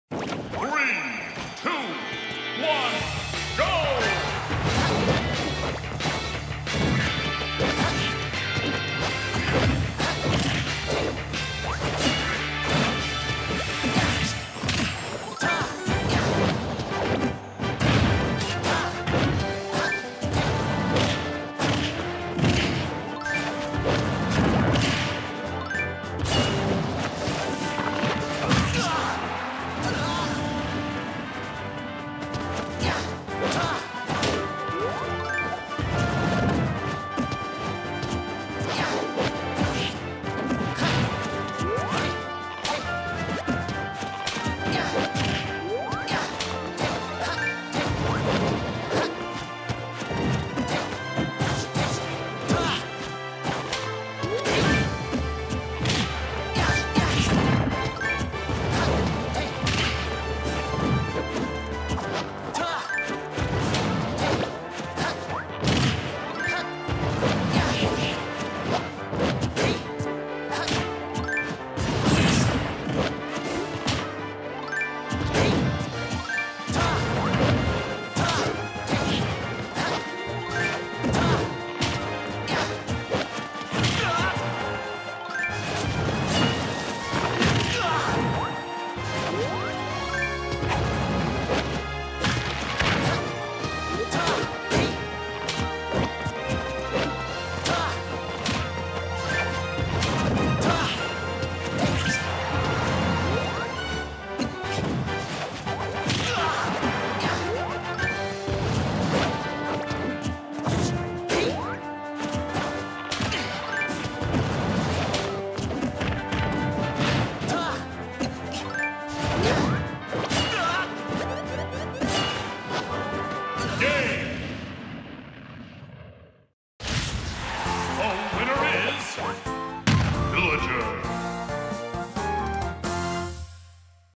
VILLAGER. JayE (Marth) vs. AceStarThe3rd (Villager)
My 2nd match against AceStarThe3rd. I had a 3rd match against him, but I forgot to save it :/ I would have went Link if I knew he would switch to Villager. Got beaten pretty good lol. oh well, good games, he's pretty good.
Villager, Marth